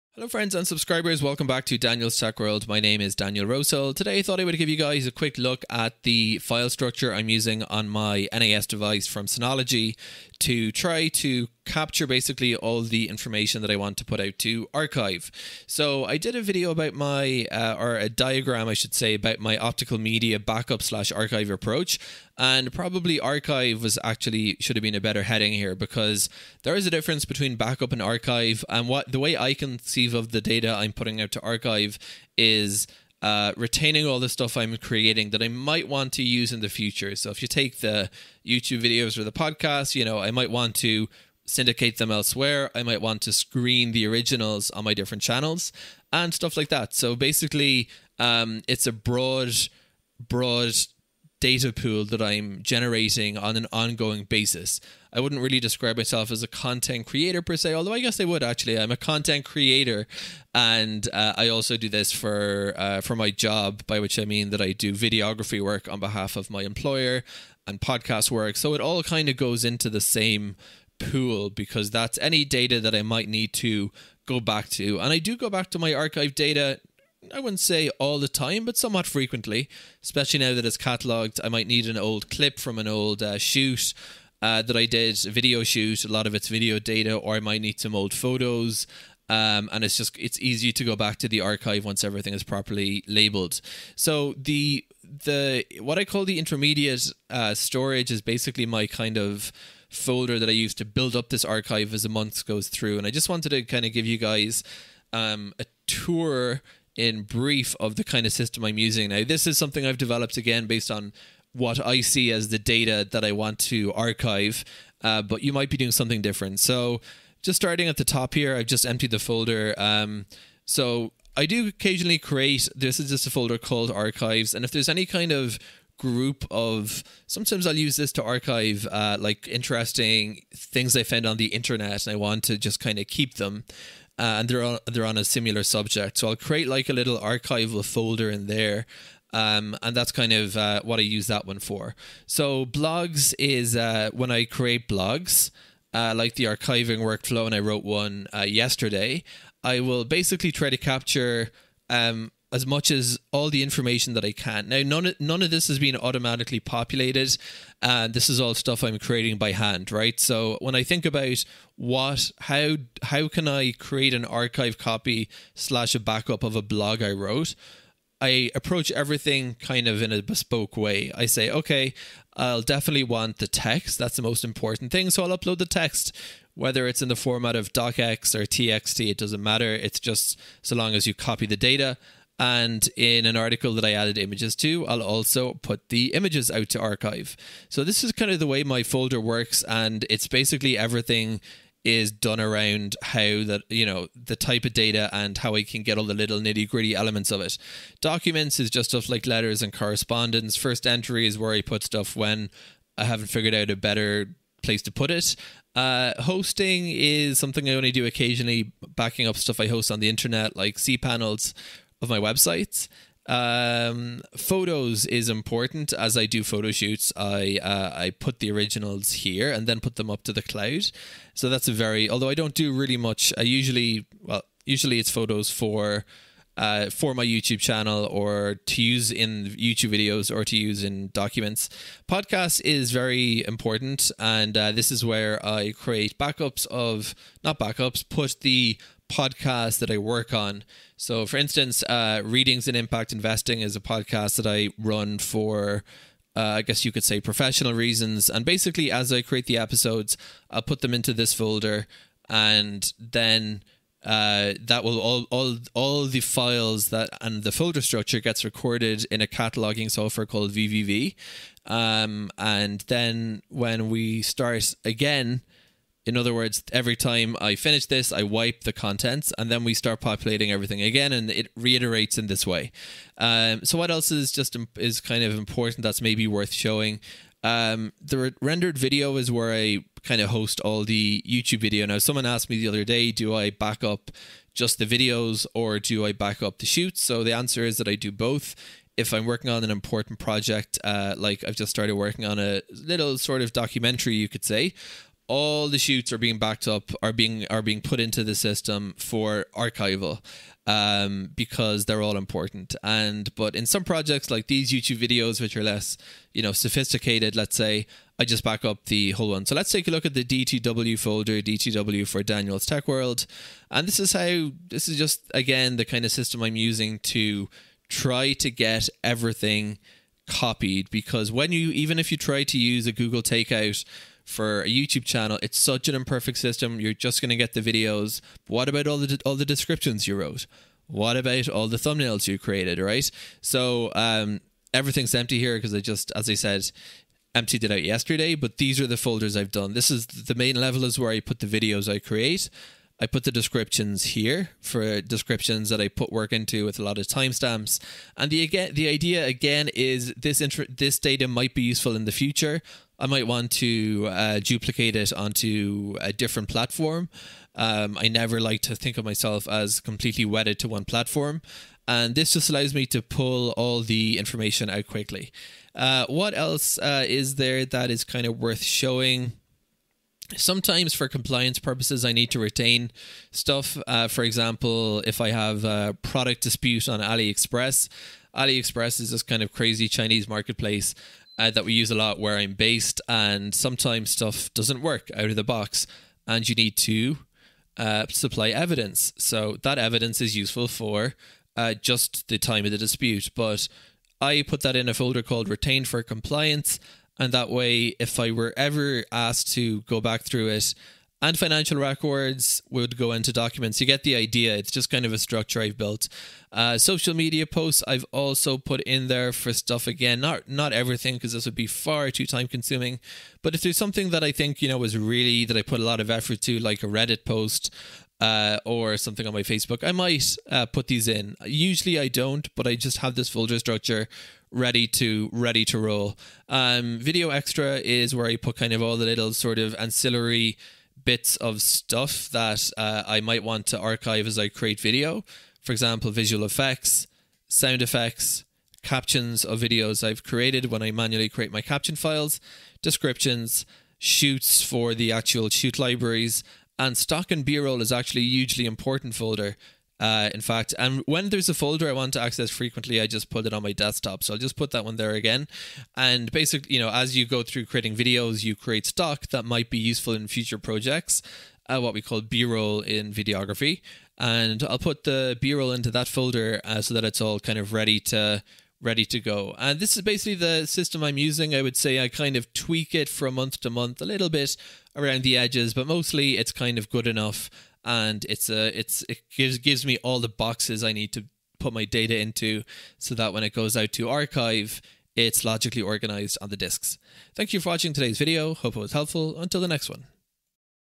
The Folder Structure I Use To Build My Digital Archives
The current folder structure I'm using to build my personal data footprint and which I then use to write material to disc for archive. Would love to hear how others organise their data!
Video: Daniel Rosehill (@DanielSRosehill)